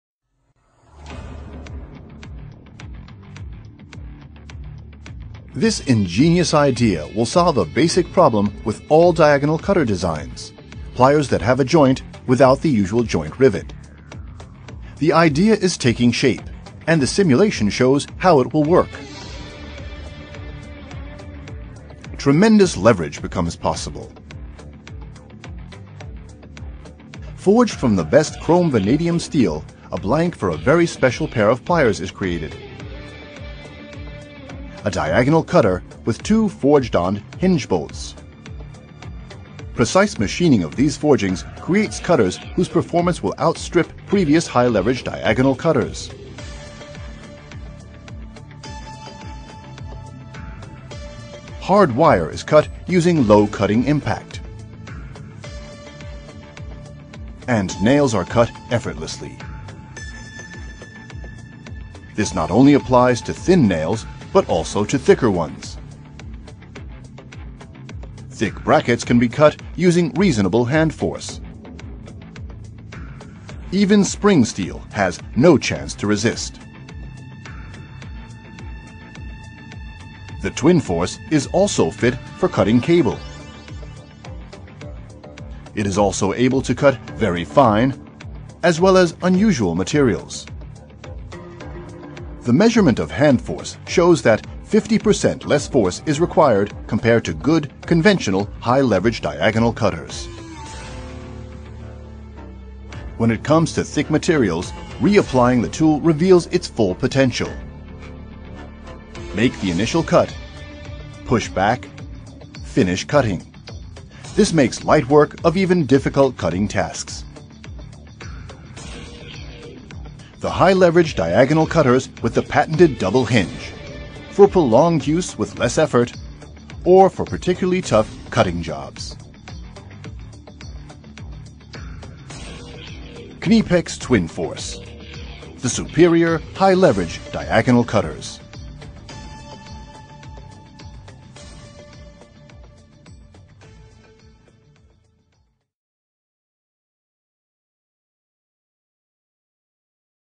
KNIPEX Szczypce tnące boczne TwinForce do cięcia drutu KAMMAR24
Szczypce KNIPEX TwinForce przecinają nawet drut o grubości 4 mm przy dwu-/trzykrotnym docinaniu bez użycia dużej siły. Tradycyjne szczypce tnące boczne o zwiększonym przełożeniu nie przecinają materiałów o takich średnicach lub wymagają użycia bardzo dużej siły. Istnieje także możliwość docinania. Optymalne przełożenie dzięki podwójnemu złączu. Bardzo łatwe cięcie przy niewielkim nakładzie siły: tną o 50% łatwiej niż sprawdzone szczypce tnące boczne o zwiększonym przełożeniu firmy KNIPEX.